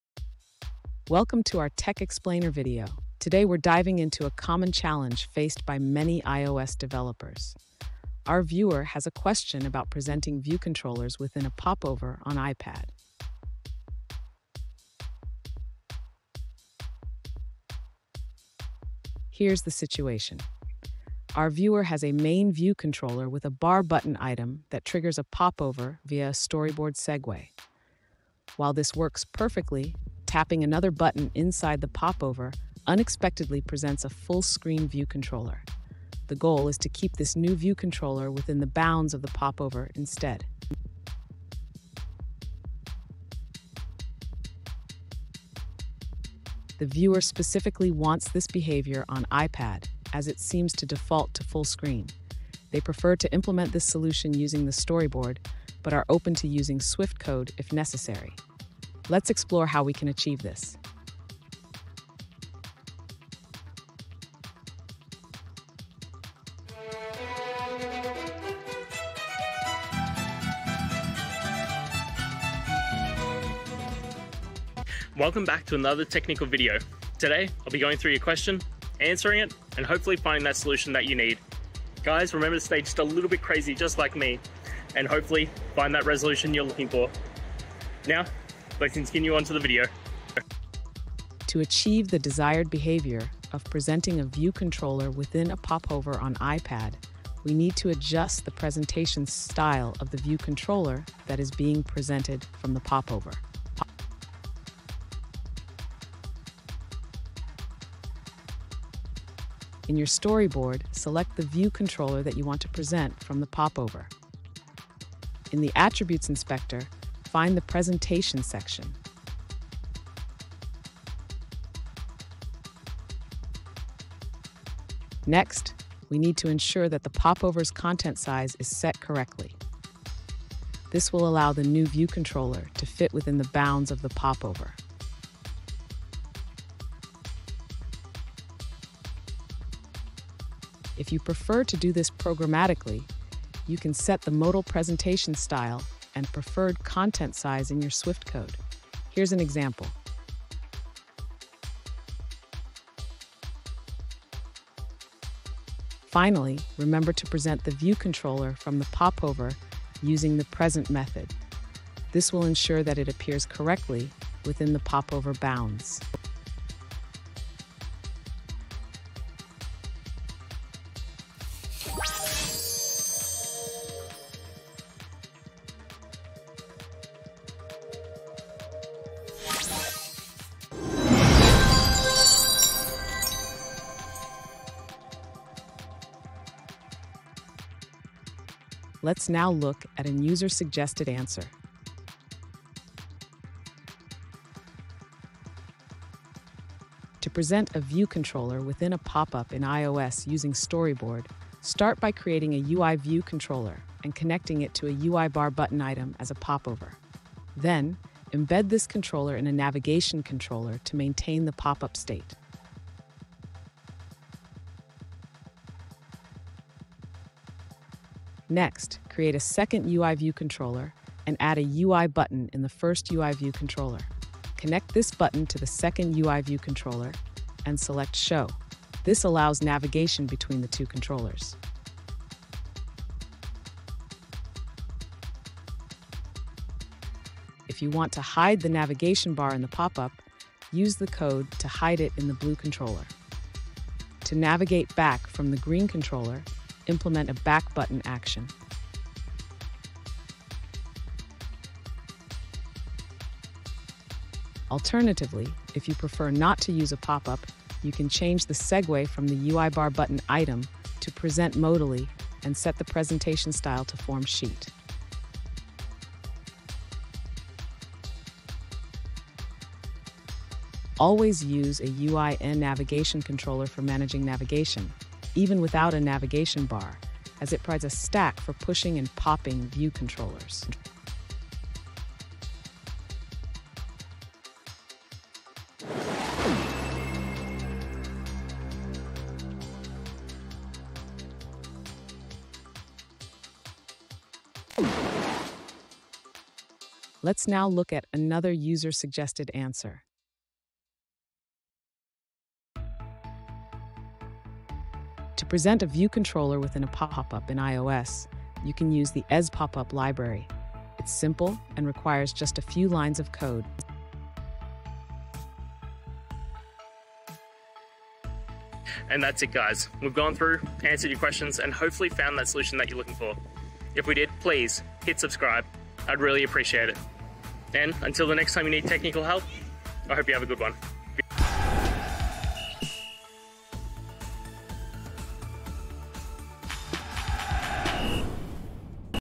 How to Present View Controller in iOS Popup: Step-by-Step Guide
In this video, we’ll explore the process of presenting a view controller in an iOS popup, a common design pattern that enhances user experience by providing context-sensitive information without navigating away from the current screen. Whether you're a beginner or looking to refine your skills, this step-by-step guide will walk you through the essential techniques and best practices for implementing popups in your iOS applications. Join us as we dive into the code and design considerations that will help you create seamless and engaging user interfaces.

Today's Topic: How to Present View Controller in iOS Popup: Step-by-Step Guide

Related to: ios, viewcontroller, popup, presentviewcontroller, iospopup, step-by-stepguide,